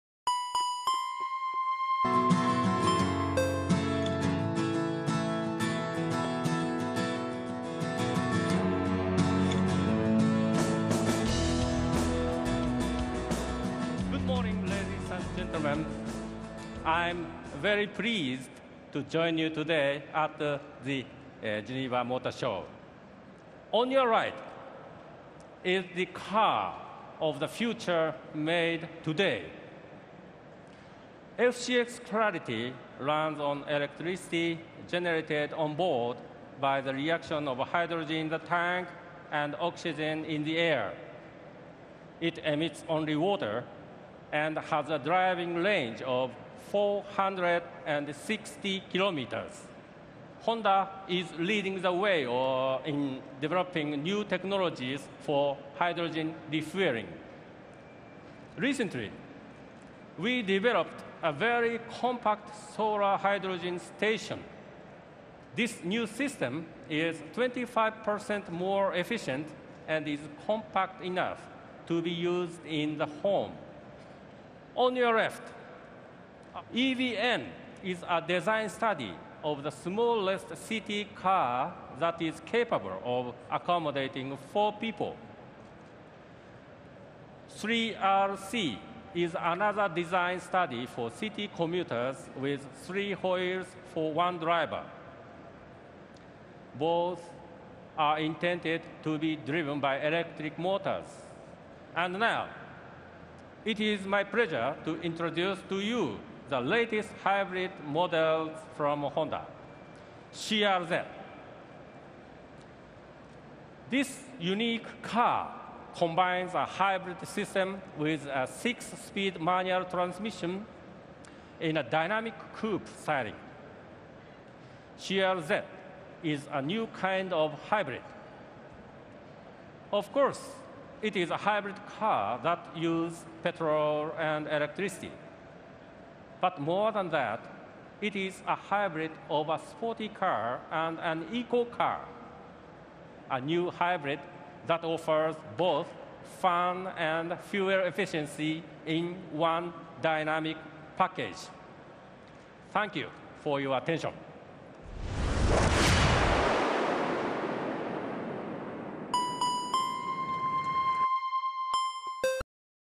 Honda Press Conference at the Geneva Motor Show
The Honda 3R-C concept makes its world debut in Geneva, alongside the EV-N concept which makes its European premier. The 3R-C, a three wheeled battery electric model, envisions a single occupant vehicle for zero emission commuting. The two-door EV-N concept is envisioned with a Lithium ion battery pack, and small electric motor for predominantly urban use. This model incorporates Honda's own solar panels in to the roof, to charge the car while it is parked.